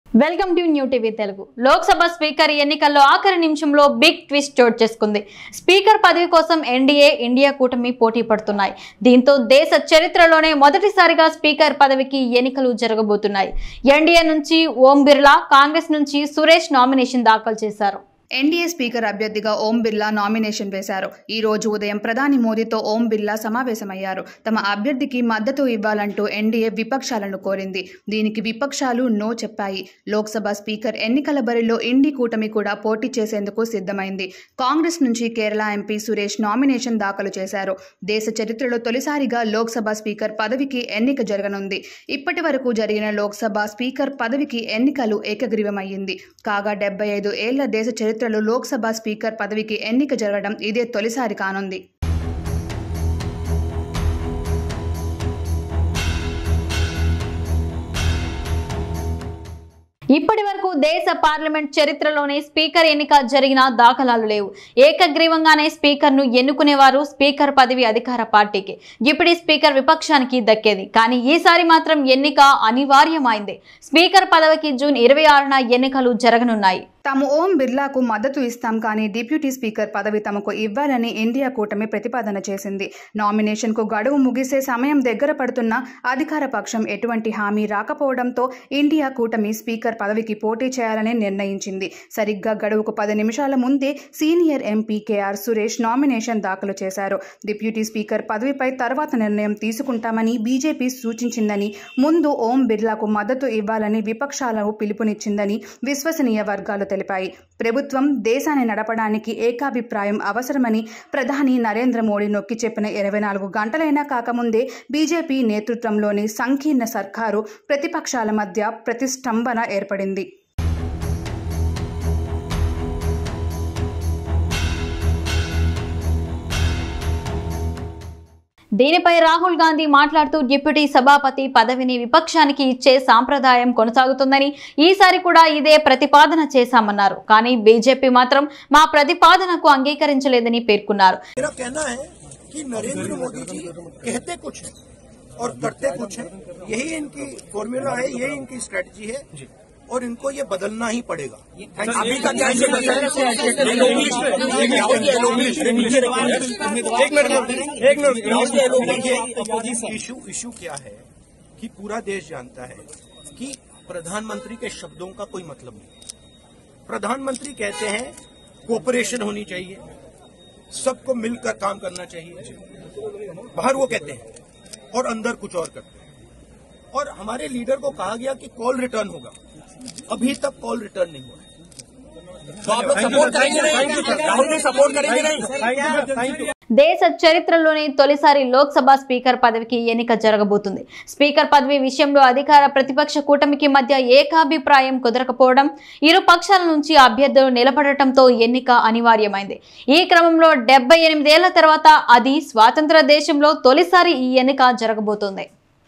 లోక్ సభ స్పీకర్ పోరులో నెగ్గేదెవరు..? - Lok Sabha Speaker Election | PM Modi Vs Rahul Ghandi
Watch► లోక్ సభ స్పీకర్ పోరులో నెగ్గేదెవరు..? - Lok Sabha Speaker Election | PM Modi Vs Rahul Ghandi